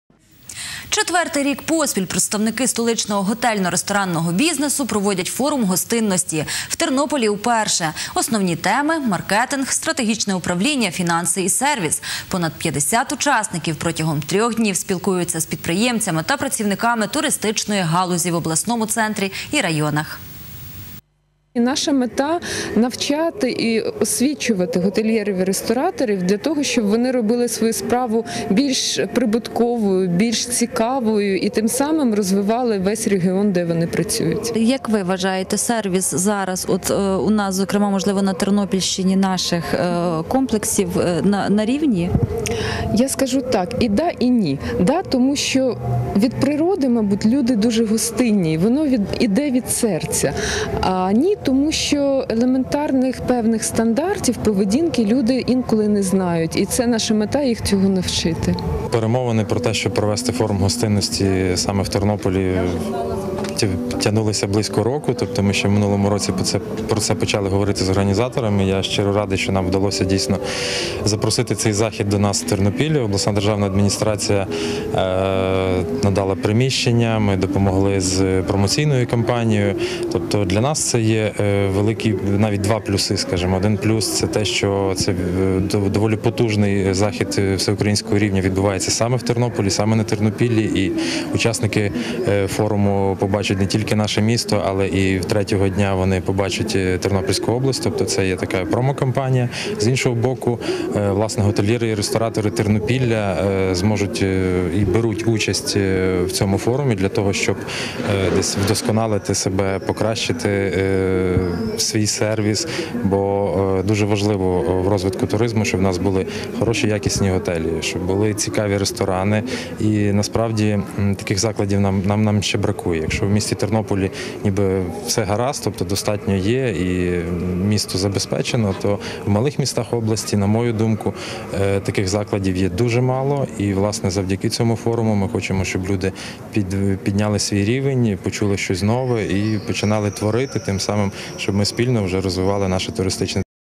Тернопіль прийняв Форум гостинності
Четвертий рік поспіль представники столичного готельно-ресторанного бізнесу проводять Форум гостинності. В Тернополі – уперше. Основні теми – маркетинг, стратегічне управління, фінанси і сервіс.